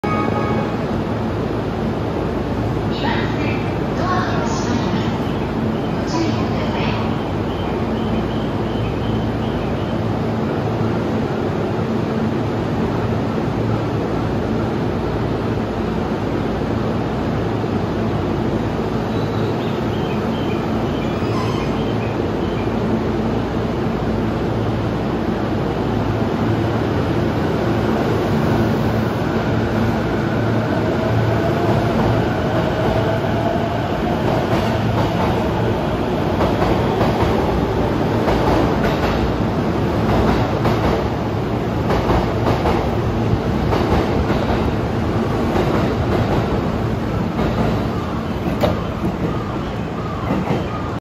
E233系1000番台ｻｲ150編成（根岸線全線開通50周年記念HM掲出）が京浜東北線快速大船行きとして大宮駅2番線を発車するシーン（1221B）2023.7.27.12:58
懐かしいな